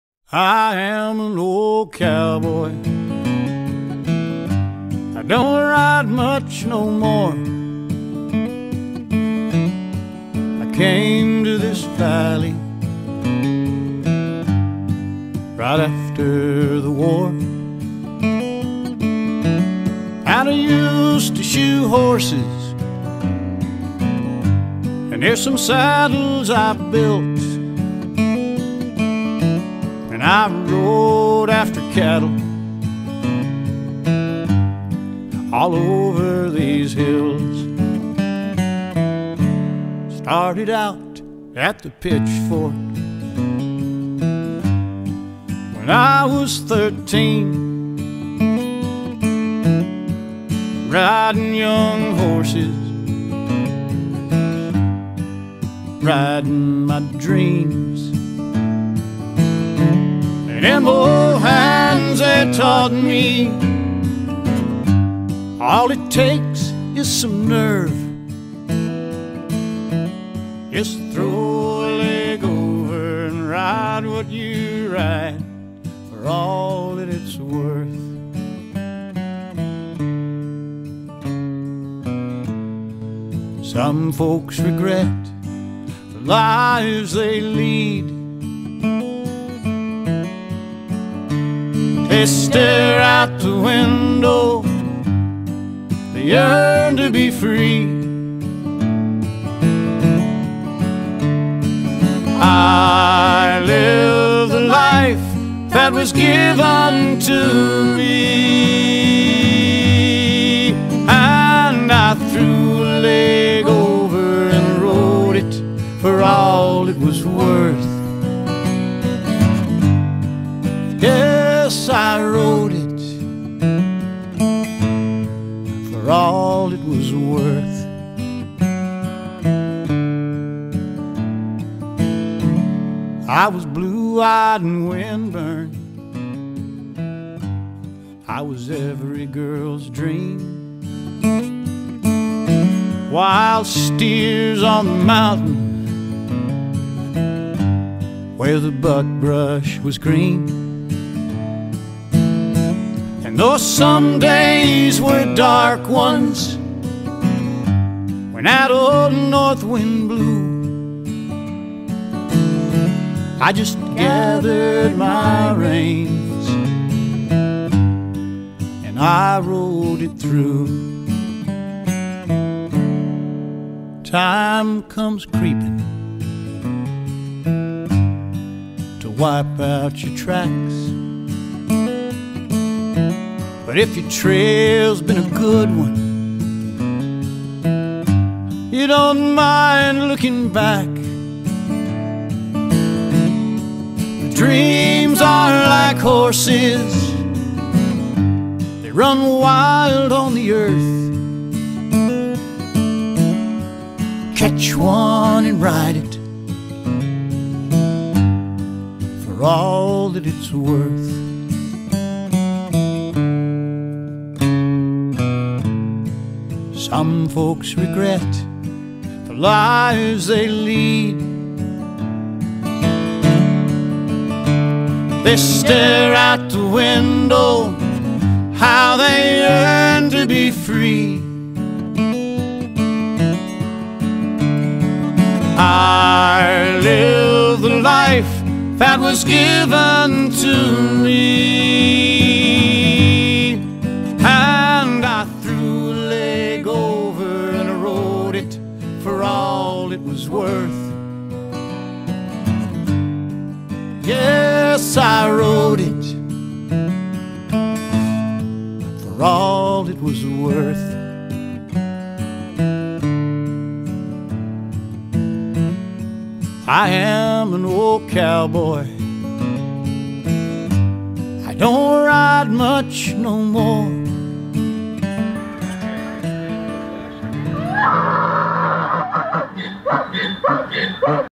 Bishop Mule Days 2016
Bishop Mule Days 2016 with a cameo by McGee Creek Pack Station and music by Dave Stamey